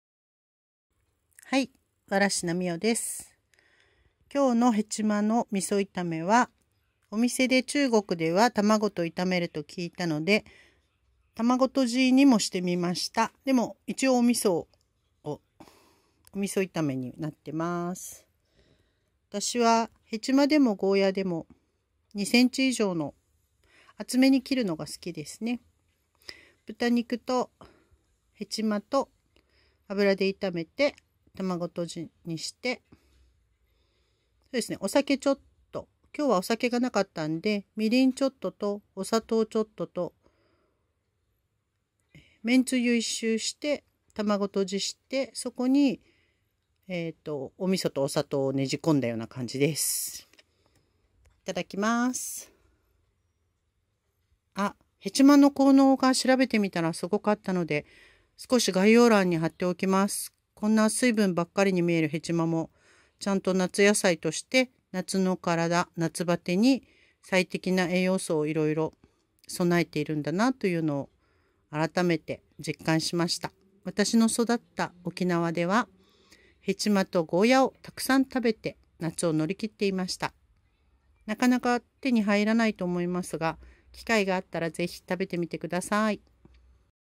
今日の一皿 ナーベラー味噌炒めの卵とじ
夏バテ防止 メニュー

私の育った沖縄では、ヘチマはたわしや化粧水などの美容原料ではなく、立派な食べ物です！  大好物です。ナスに近いと言えば近いかなー。ナスでもなく、キュウリでも、ウリでもなく。へちま独特の風味とへたれな食感。

本日は、お店のかたに聞いて卵とじも取り入れてみました！

効能が、、覚えきれません！
ビタミンとにかくたっぷり！！
ビタミンKだって。あと葉酸とサポニン。

んで、整腸⇒便通⇒美肌効果。
解毒効果も、去痰作用、咳を鎮めるとかもあったな。
コレステロール除去、肥満防止、
血行促進、生活習慣病予防、免疫力アップ、
なんかいいことづくめじゃない？